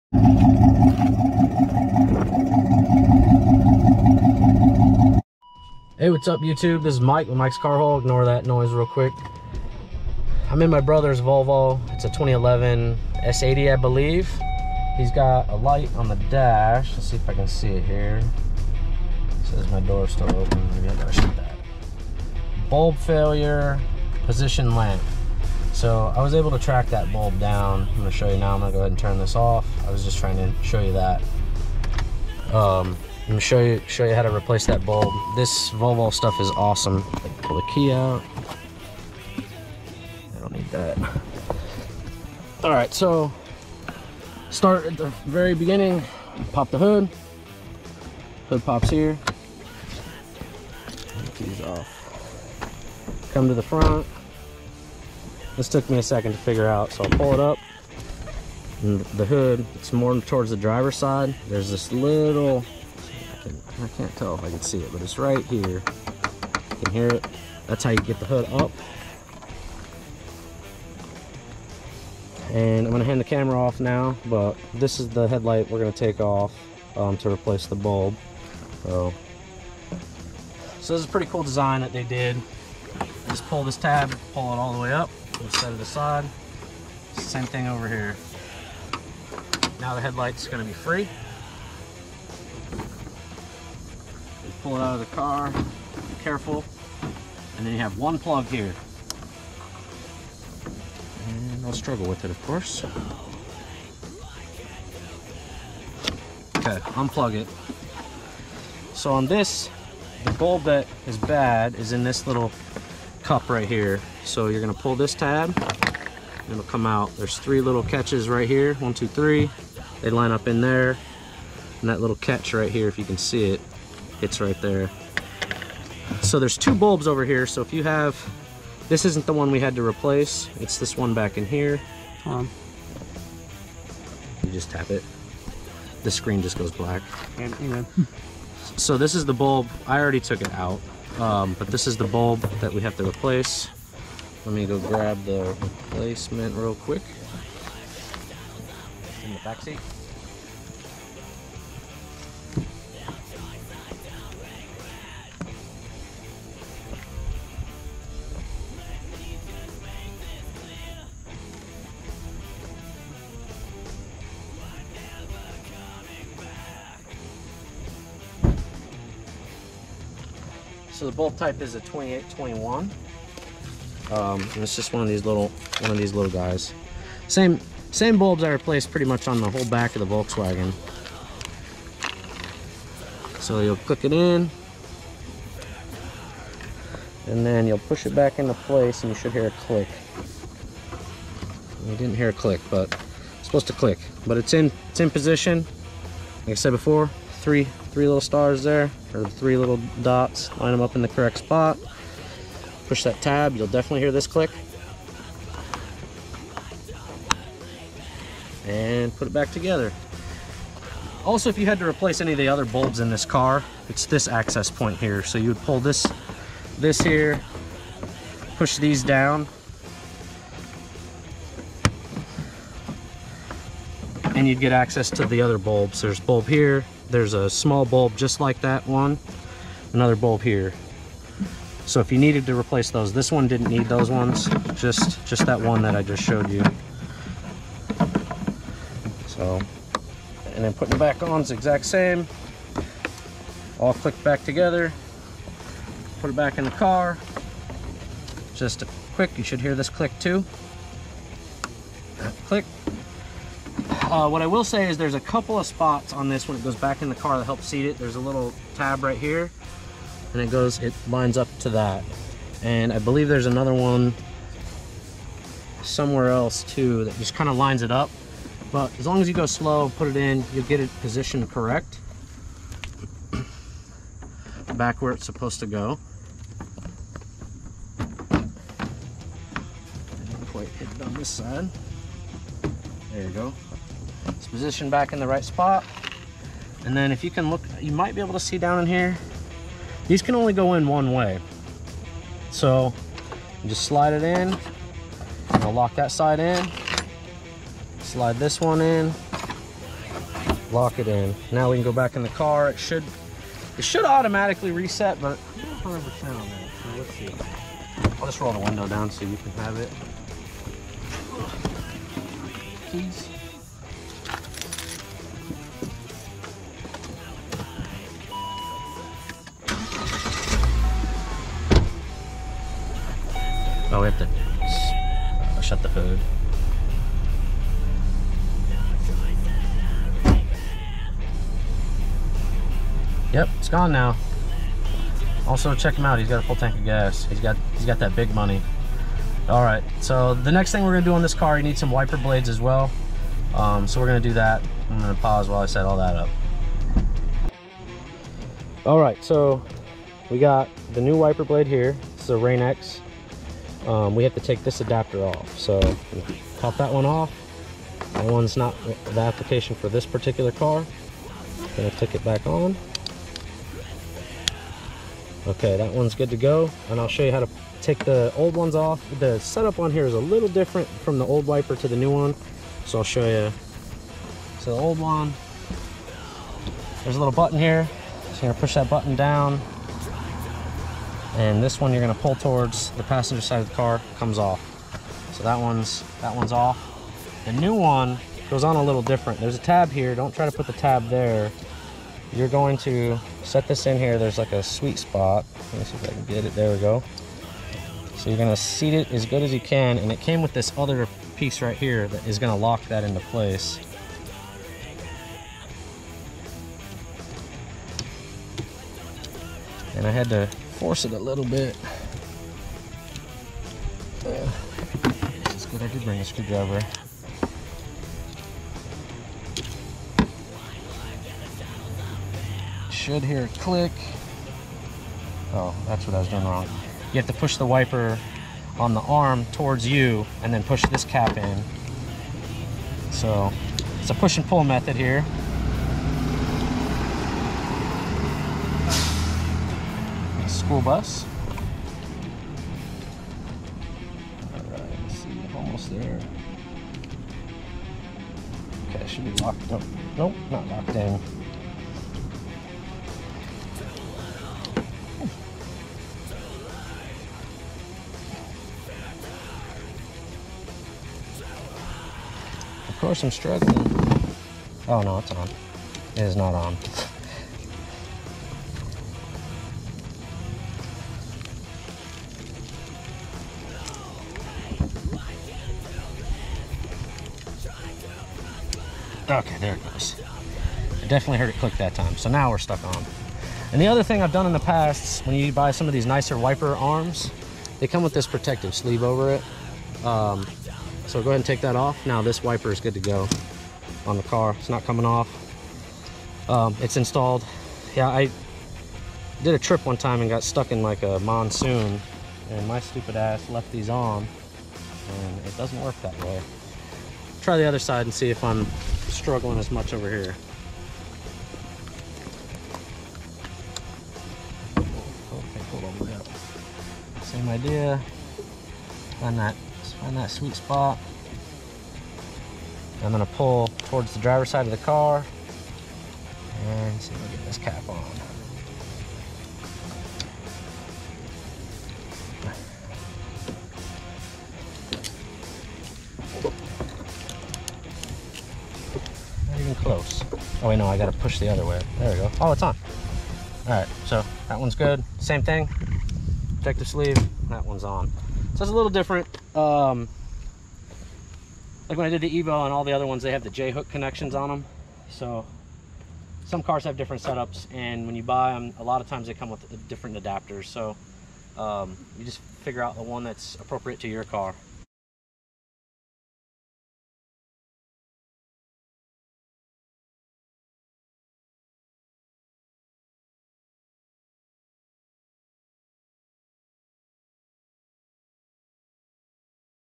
How to headlight bulb replacement/How to Replace Windshield Wipers on Car
Changing your headlight bulbs is an easy form of maintenance that you can perform on your vehicle. Watch as is how you how to install new bulbs in your vehicle. Replacing your car, truck, or SUVs headlight bulbs can help increase your nighttime visibility. this is for the 2011 S80 Volvo i was super impressed how easy this really was. I have not doubt everyone of my viewer can accomplish this as well.

How to replace wiper blades. Although it seems trivial, watching how to install wiper blades is useful because when it is dark, pouring rain, snowing, or thunder and lightening out, you do not want to have to learn how to put wiper blades on. This  video will show you how to put a wiper on.